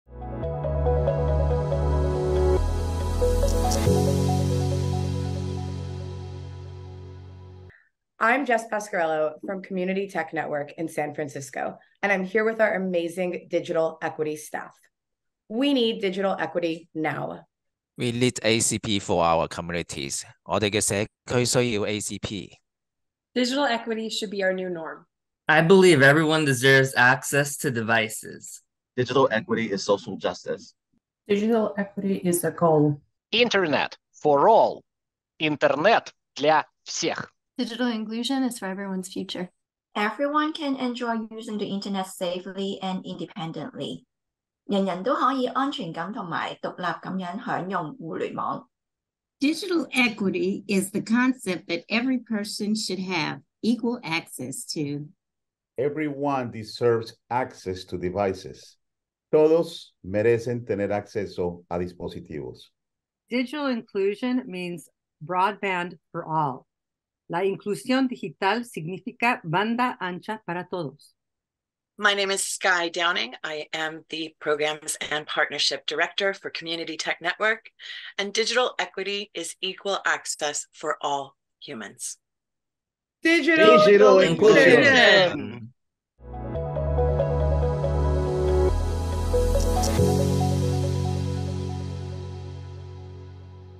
What Digital Equity Means
Hear from some of CTN’s staff about what digital equity and inclusion means to them and why it is so important.